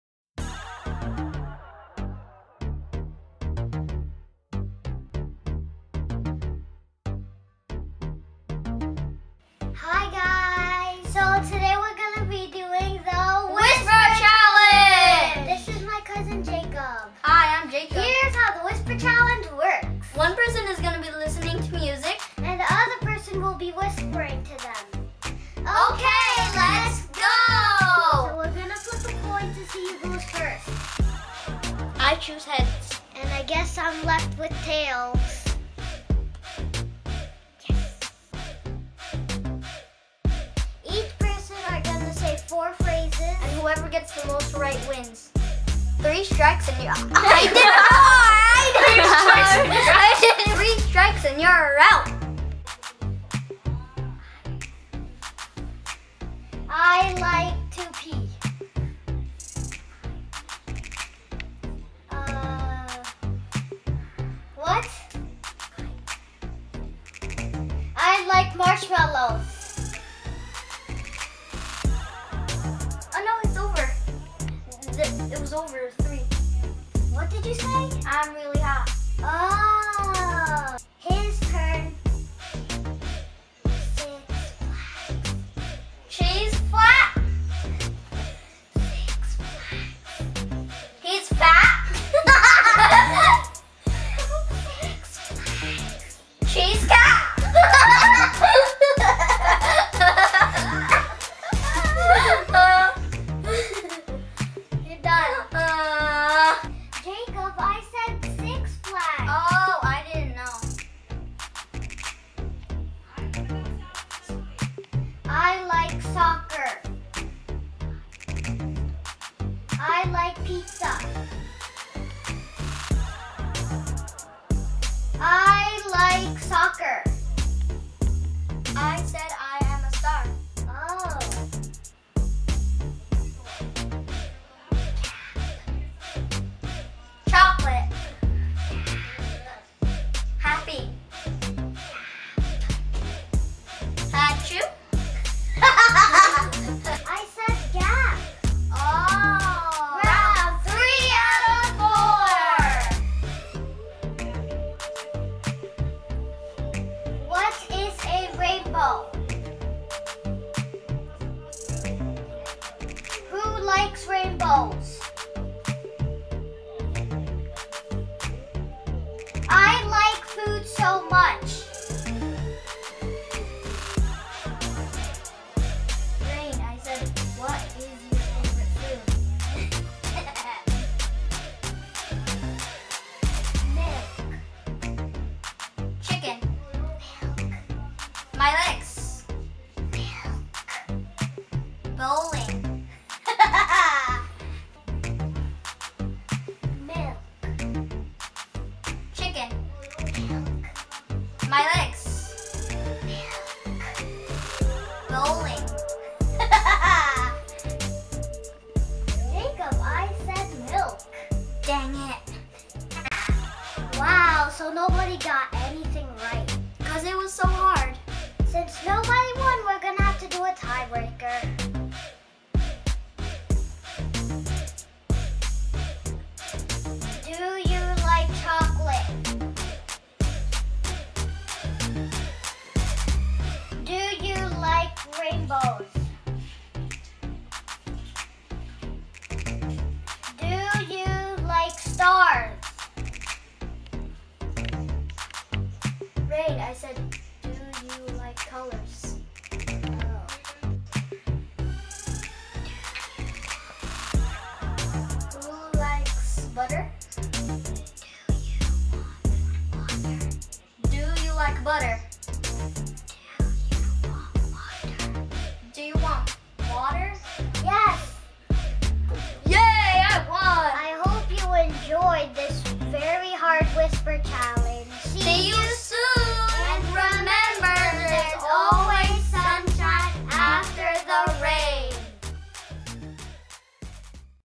Whisper Challenge with Jacob!
Here's the WHISPER CHALLENGE I did with my cousin, Jacob! Whew!